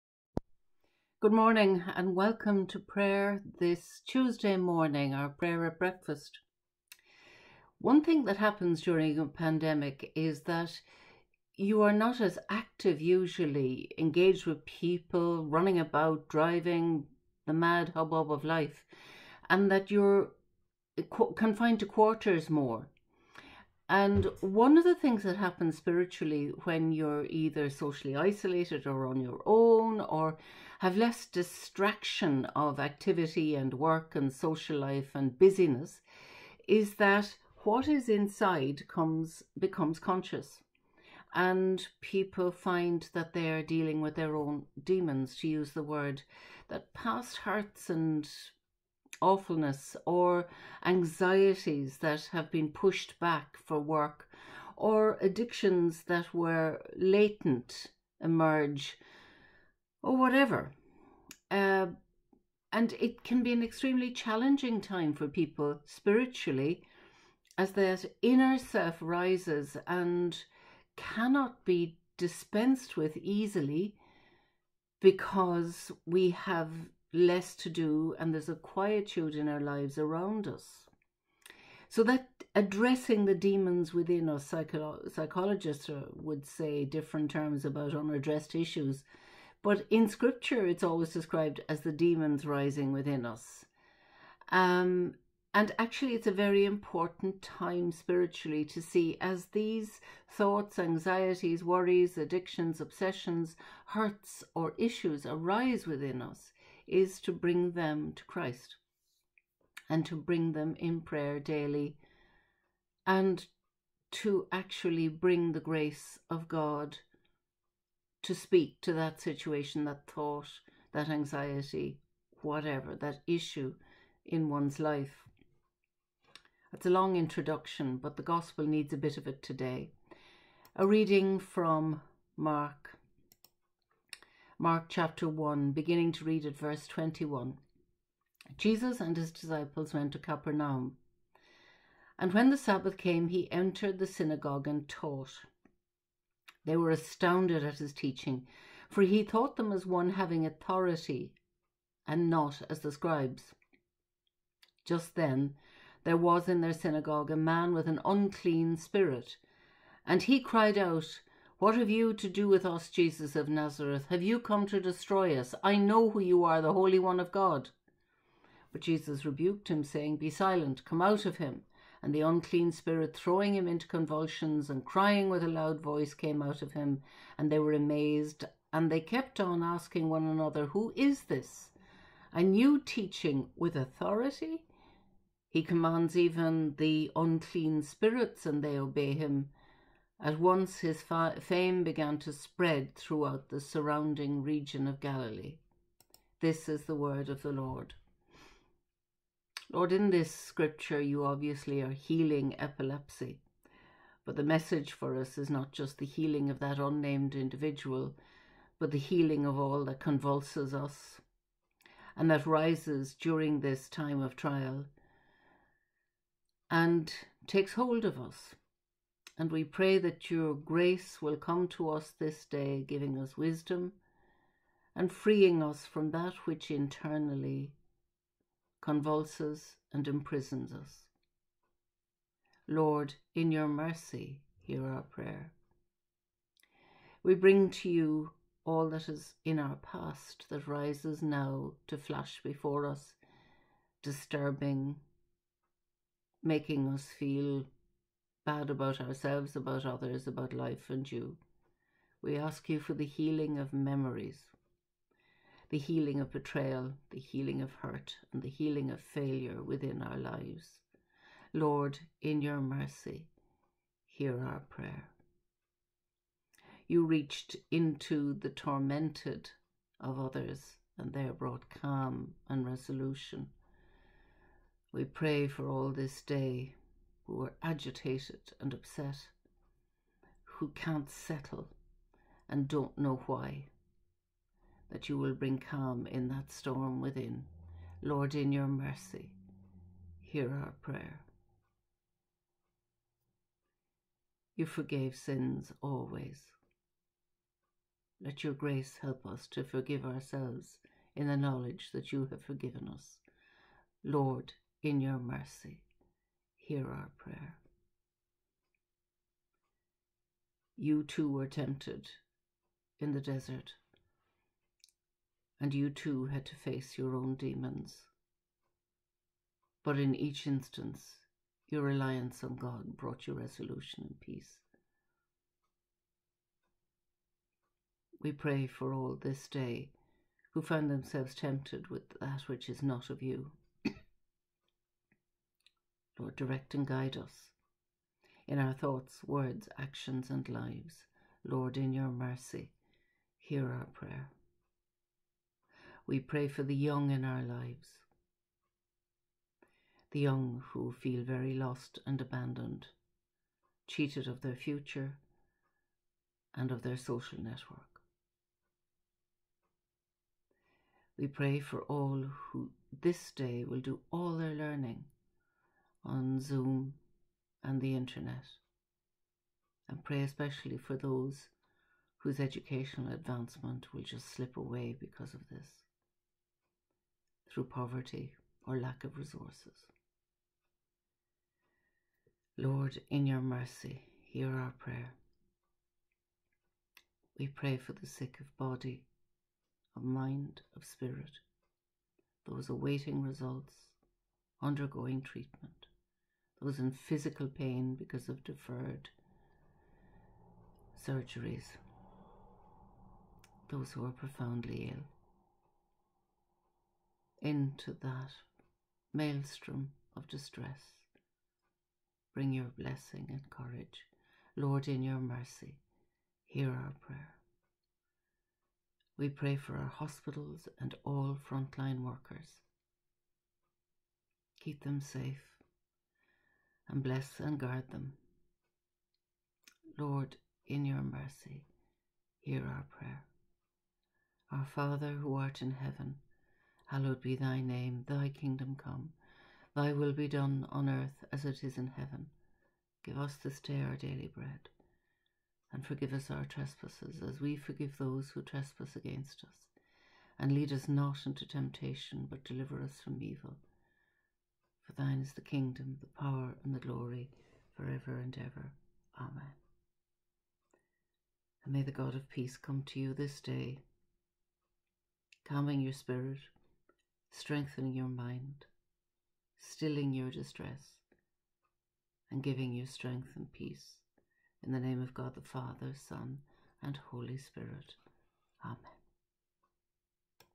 12.01.21 Prayer at Breakfast
21 They went to Capernaum; and when the sabbath came, he entered the synagogue and taught. 22They were astounded at his teaching, for he taught them as one having authority, and not as the scribes. 23Just then there was in their synagogue a man with an unclean spirit, 24and he cried out, ‘What have you to do with us, Jesus of Nazareth? Have you come to destroy us? I know who you are, the Holy One of God.’ 25But Jesus rebuked him, saying, ‘Be silent, and come out of him!’ 26And the unclean spirit, throwing him into convulsions and crying with a loud voice, came out of him. 27They were all amazed, and they kept on asking one another, ‘What is this? A new teaching—with authority! He* commands even the unclean spirits, and they obey him.’ 28At once his fame began to spread throughout the surrounding region of Galilee.